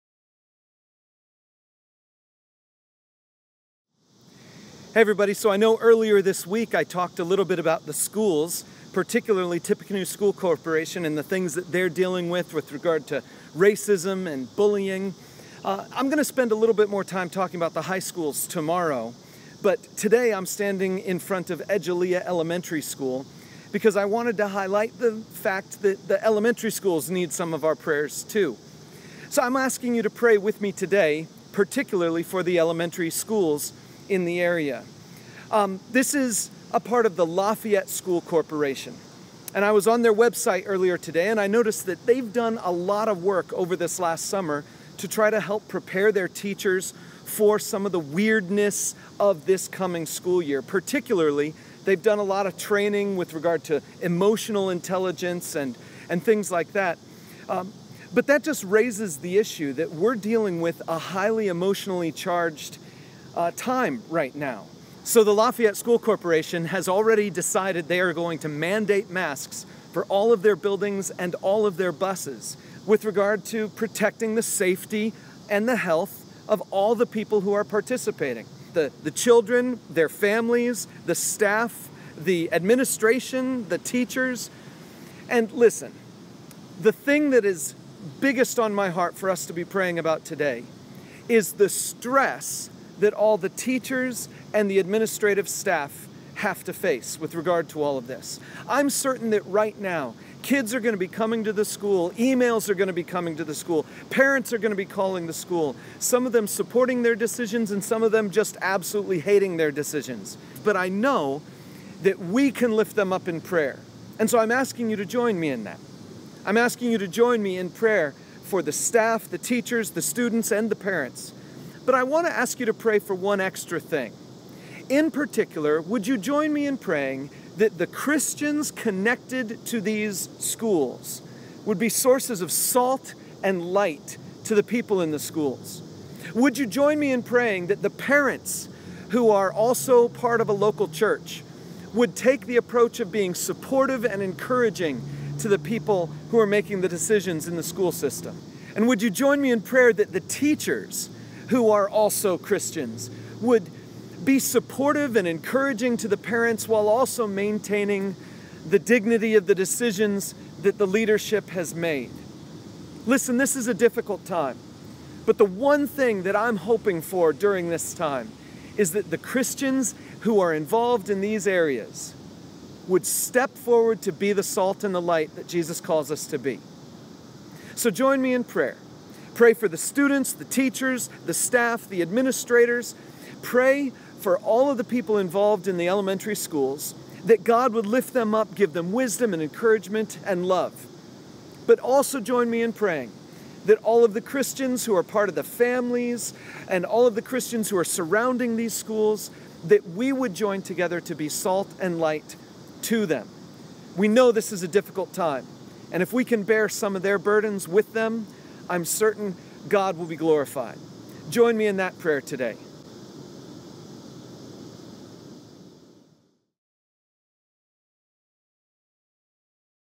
21 Days of Prayer - Day 19 - Elementary Schools
The elementary schools around Lafayette are facing a rough start to the year. Specifically, in the LSC, they have decided to require masks for all people in buildings or busses regardless of vaccination status. That decision has some parents pretty upset, and the teachers and administrators are trying to address it with grace and wisdom. Let's specifically pray that the Christians involved in all of this would be sources of salt and light to the parents, teachers, staff, and administrators so that God would be glorified.